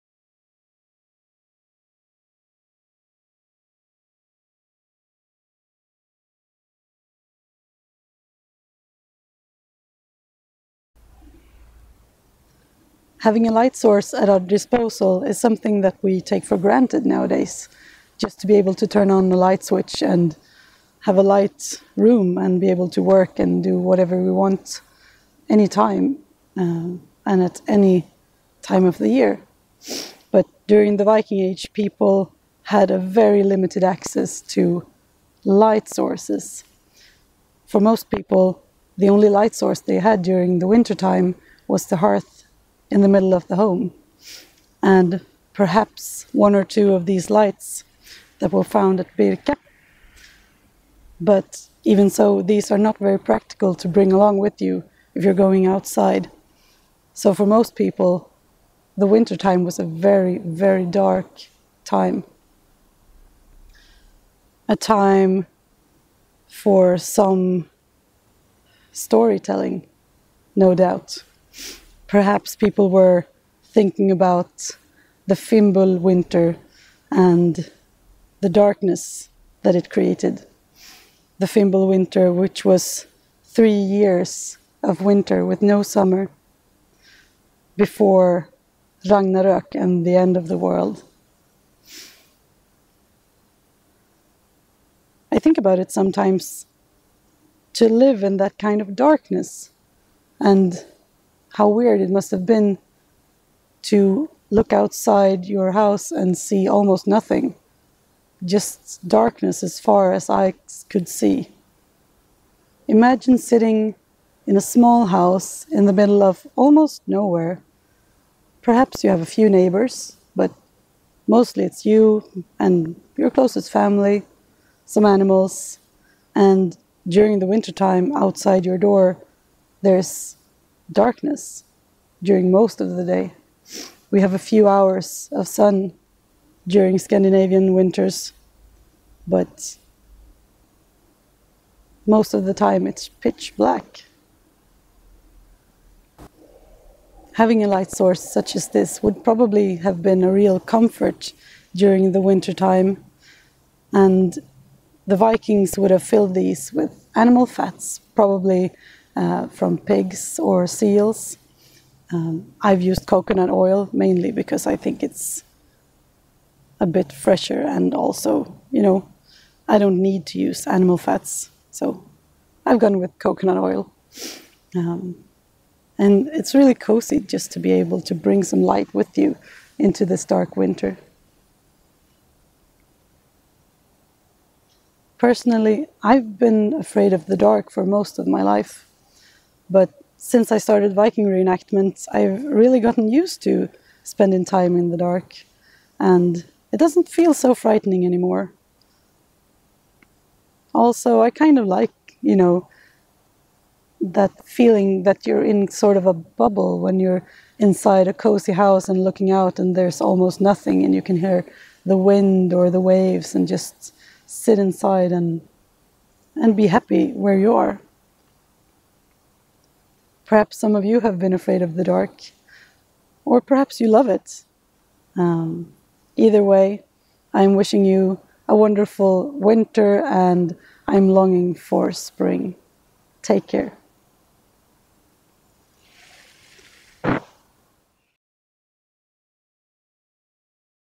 Viking age light sources | Living in the dark
What was it like to live through the dark Scandinavian winters during the viking age?

The lamp in the video was made by Alexander Krohn and can be found